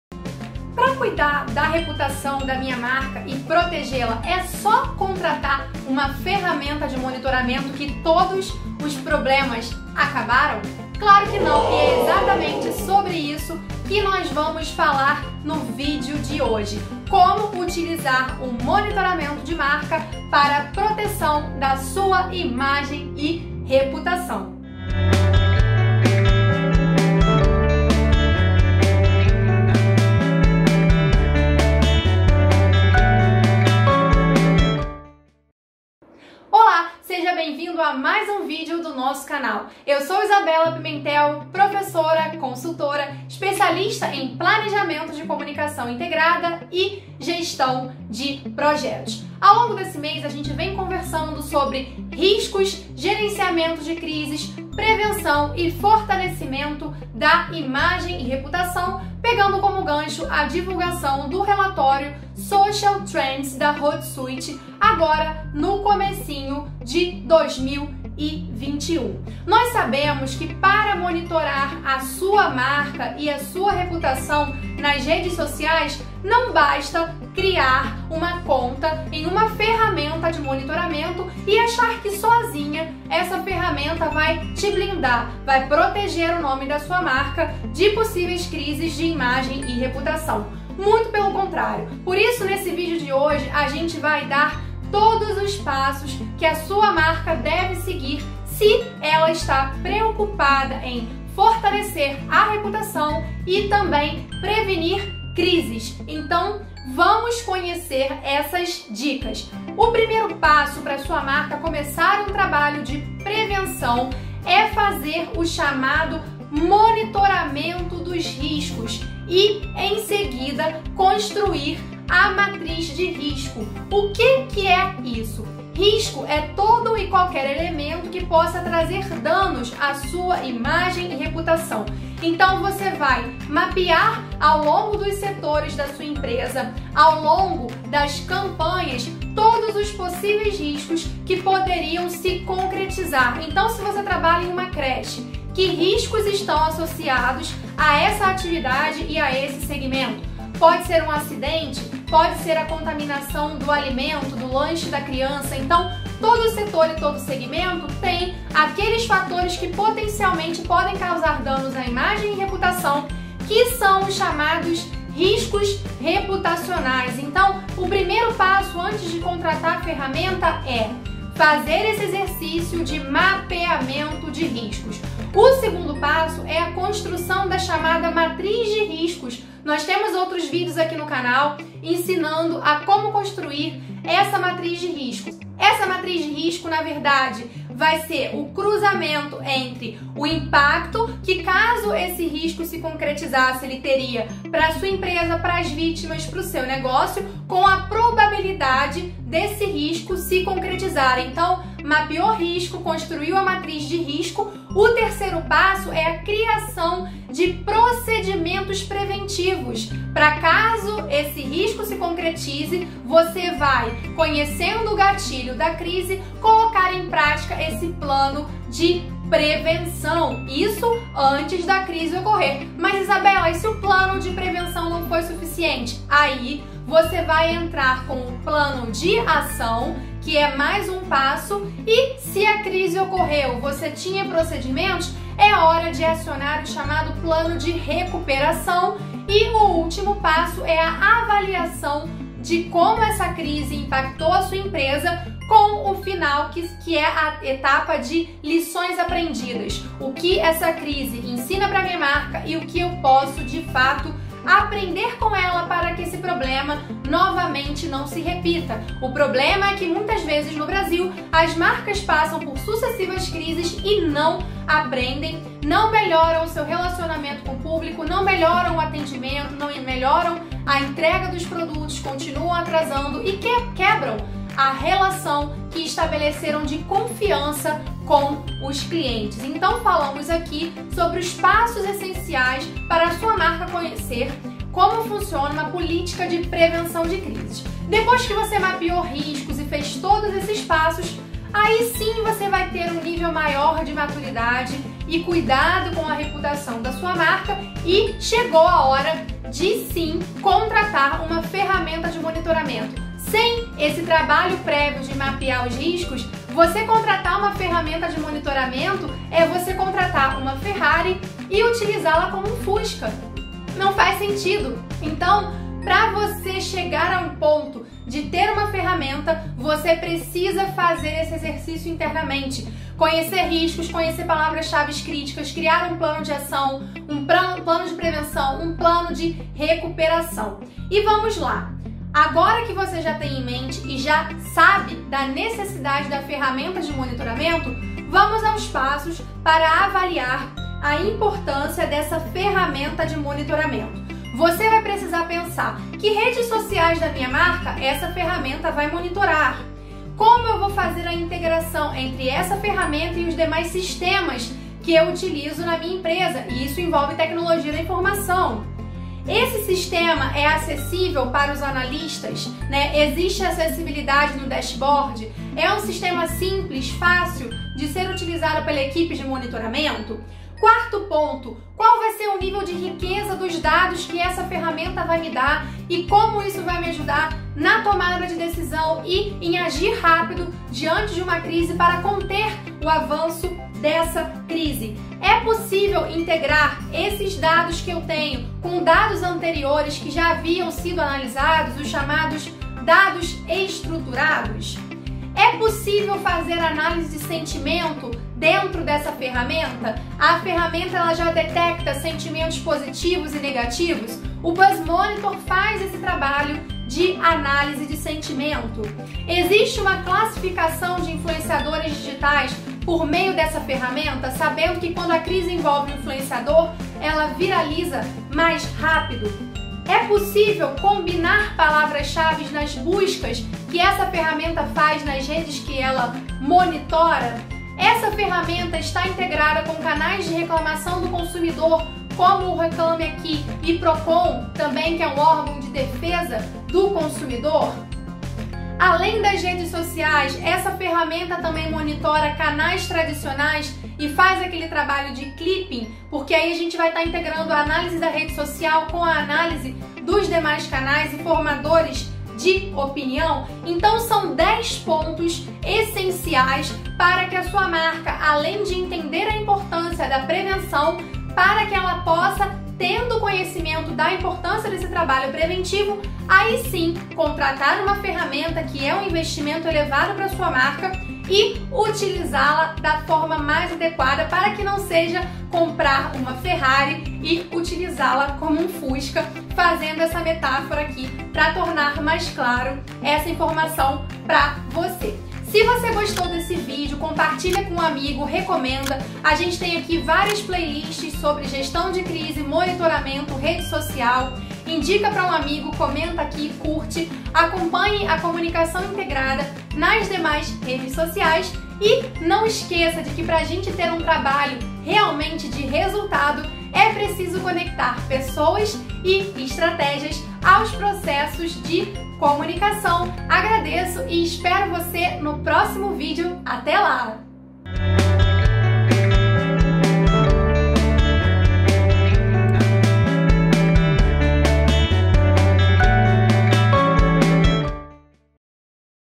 Gestão de riscos reputacionais: 10 dicas para as redes sociais da sua marca
Mudança de uma postura de ‘escuta social’ (social Listening clássico) para uma comunicação data driven e não interruptiva (marketing baseado na oportunidade e na permissão, não no oportunismo).
Usar a escuta social como uma ferramenta de inteligência, ajudando as organizações a tomar decisões mais inteligentes com base nas necessidades e pontos problemáticos dos clientes.
Quando não há dados concretos, o silêncio é de ouro
Um dos capítulos que mais chama atenção do relatório 2021 é Silence is Golden (o silêncio é dourado, em livre tradução).

Diante do aumento dramático do uso das redes sociais e do enorme número de novas oportunidades para as marcas este ano,  muitas delas agiram prematuramente, pegando carona em causas que não abraçam e se posicionamento diante de temas sobre os quais não tem atuação ou propriedade.

 Monitore para prevenir
Monitoramento é essencial nos dias de hoje. Pequenas críticas e comentários negativos podem viralizar rapidamente para outros canais, afetando a reputação da sua empresa.

Mas, antes mesmo de contratar uma ferramenta de monitoramento,  é preciso fazer uma série de atividades, pois não basta monitorar sem que isso esteja alinhado à uma política de mapeamento de riscos e a uma cultura preventiva.

O primeiro passo antes de contratar uma ferramenta é construir um mapa de risco da sua empresa, ou seja, uma lista das vulnerabilidades que podem gerar eventos negativos à marca, considerando o impacto e probabilidade de ocorrência.

Depois que temos em mão o mapa de riscos e dados estruturados da análise do macro e microambiente, é hora de ver quais são as palavras-chave mais recorrentes nessa lista, e daí extraímos o que precisa ser, de fato, monitorado, para que um comentário negativo não viralize.

Mapa de riscos reputacionais
Tendo em mãos o mapa de risco e a lista de palavras mais críticas, o ideal é construir um protocolo de gestão de crise em que estejam previstas  perguntas e respostas para possíveis dúvidas e comentários que sejam postados mencionando a marca. Essas diretrizes não devem ser criadas para ficar na gaveta, então, compartilhe-as com toda equipe, do social media até o assessor de imprensa.

Vamos ao monitoramento?
Mapa, protocolo e treinamento com a equipe, agora sim, vamos ao mercado pesquisar as melhores ferramentas. Particularmente, consideramos os seguintes requisitos antes de recomendar uma ferramenta ao cliente:

Redes que essa ferramenta monitora;
Integração na hora de gerar relatórios;
Acessibilidade do dashboard;
Riqueza de dados dos relatórios;
É possível integrar os dados não estruturados com CRM?
Há análise de sentimento?
Existe classificação dos influenciadores por tema;
Torna-se possível monitorar combinação de palavras-chave;
Se a ferramenta puxa dados e menções do Reclame Aqui e outra plataformas de relacionamento com consumidor;
Já mapeou os riscos? Conta para a gente e vamos trocar experiências!